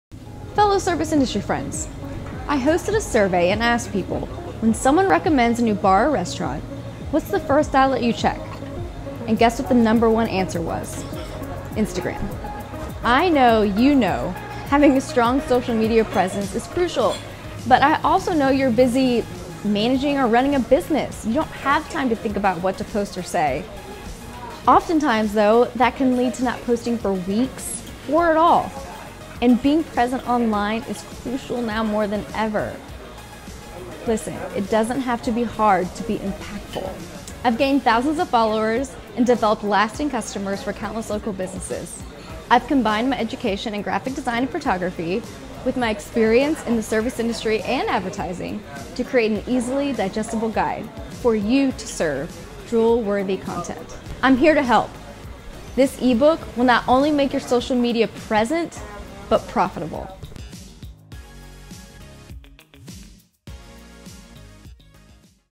Jess Amburgey "Serving Social Media"
Jess Amburgey tells us more about her new E-book "Serving Social Media"